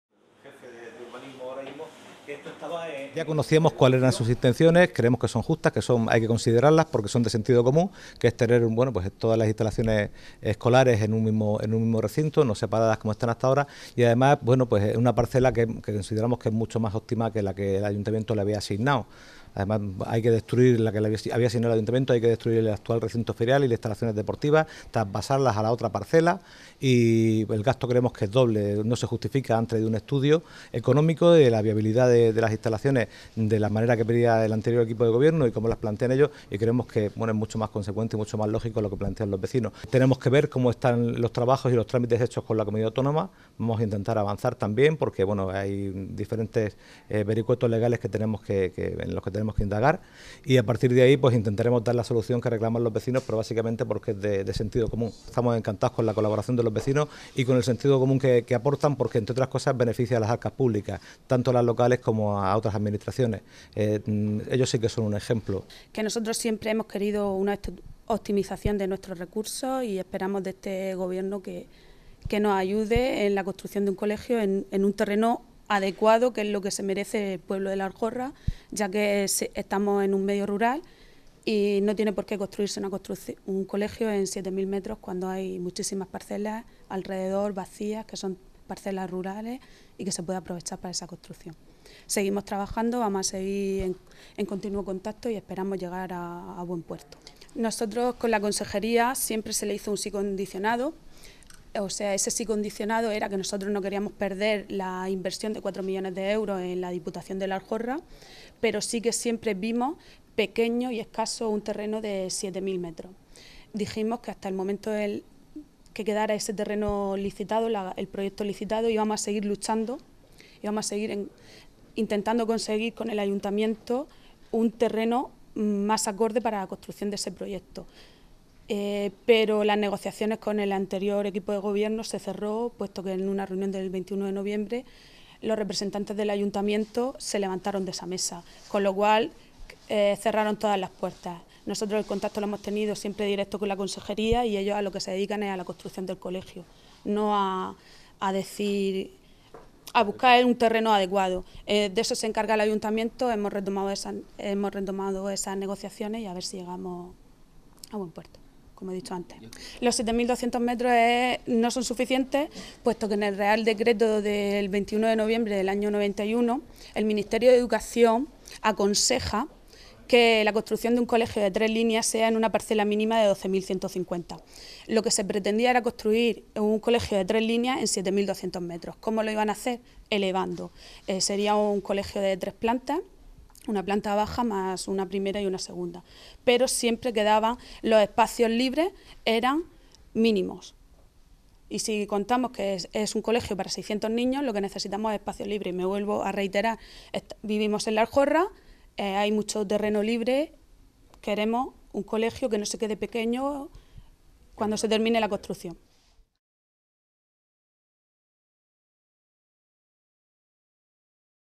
El Ayuntamiento considera justas las reivindicaciones de los padres de La Aljorra
El alcalde de Cartagena, José López, junto a la vicealcaldesa, Ana Belén Castejón, y el concejal de Cultura, Educación e Igualdad, David Martínez, se ha reunido esta mañana en el Edificio Administrativo, con miembros de la Plataforma Pro Colegio Único de La Aljorra.

Al término del encuentro comparecía ante los medios el alcalde y la portavoz de la plataforma Conchi Gómez. José López ha calificado como justas y de sentido común las reivindicaciones de los padres y madres de alumnos que quieren sustituir los dos viejos colegios del pueblo con la construcción de un solo centro, situado en un terreno amplio y adecuado.

López ha criticado que la idea propuesta por el anterior Equipo de Gobierno de hacerlo en una parcela insuficiente de 7.200 metros cuadrados, que conllevaría un costoso traslado del actual recinto ferial.

El alcalde ha señalado que se verá cómo están los trámites con la Comunidad para la construcción de este nuevo centro y a partir de ahí se intentará hacer las cosas con el sentido común que proponen los vecinos.

Conchí Gómez ha resaltado que salían de la reunión con buenas sensaciones, ya que se les había escuchado e iban a realizar un estudio exhaustivo de la propuesta.

La portavoz de la plataforma ha asegurado que en un zona rural como La Aljorra existen muchos terrenos y que la ubicación planteada por el anterior Equipo de Gobierno desoye las recomendaciones del Ministerio de Educación que para un colegio de tres líneas, como el que se va a realizar, debería utilizarse una parcela mínima de 12.150 metros cuadrados.

El centro albergará unos 600 alumnos y los espacios libres para ellos son insuficientes si no es busca otra parcela, según la plataforma, para lo que ellos proponen como alternativa una parcela de unos 14.000 metros cuadrados donde se encuentran las instalaciones deportivas de La Aljorra.

OTRAS CUESTIONES

A preguntas de los periodistas, el alcalde se ha referido al pacto de Gobierno en la Comunidad Autónoma entre Ciudadanos y el Partido Popular y ha dicho que tendrá la obligación de hablar con el Gobierno Regional que salga para plantear las reivindicaciones de Cartagena suscritas por Movimiento Ciudadano, PSOE y Cartagena Sí Se Puede.

Igualmente ha hablado de las obras de remodelación del puerto que está pendiente de una reunión con la Autoridad Portuaria, tal y como quedó ayer con su presidente, y en el que se planteará el despeje del frente de la Muralla.

Por último ha contestado a las críticas realizadas por el portavoz del grupo popular sobre la visita realizada al Pabellón de Deportes con medios de comunicación para denunciar las deficiencias de esta obra.